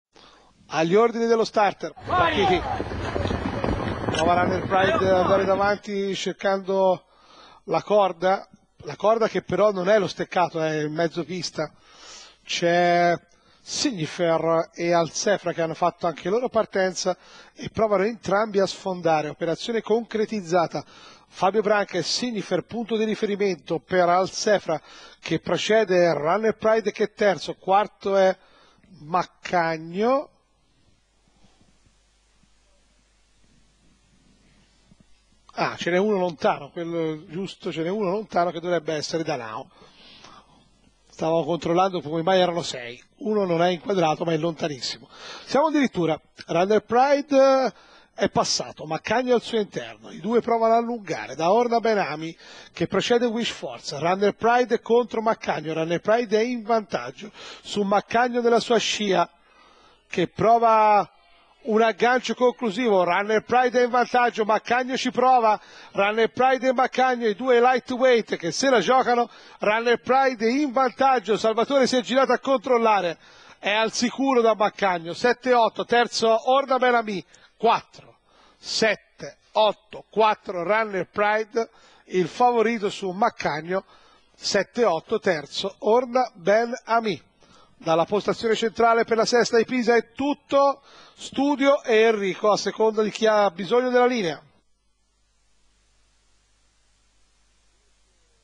04/04/2013, Pisa , Premio Piero Gargani
1. RUNNERPRIDE (SULAS SALVATORE), 2. MACCAGNO (SANNA MARIO), 3. ORNA BEN AMI (COLOMBI CLAUDIO), 4. SIGNIFER (BRANCA FABIO), 5. ALSEFRA (BOSSA FEDERICO), 6. WISH FORCE (LANDI STEFANO), 7. DANAO (MANIEZZI LUCA)